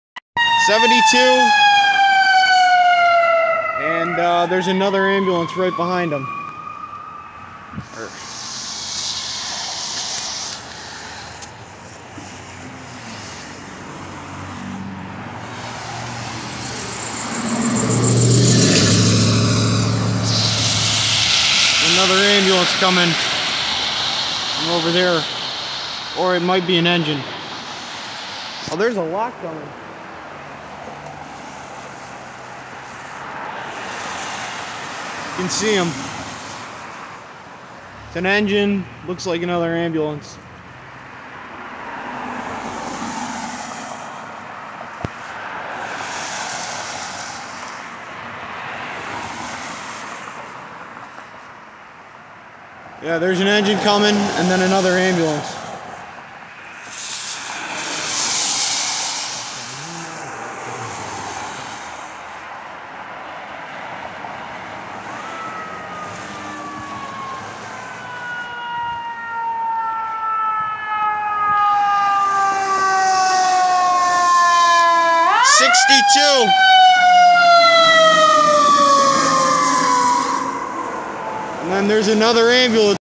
Engine 62 and ambo 72 transporting from a collapse
engine 62 and ambulance 72 transporting a victim to Christ hospital after a collapse at an off-track betting facility. the collapse occurred when heavy snow buildup on an awning dropped on one man engine 62 and truck 27 were the first to respond and truck 27 cut the Man out from below with a saw then ambulance 72 coming from engine 120s house an engine 62 transported the victim to Christ hospital where he later died.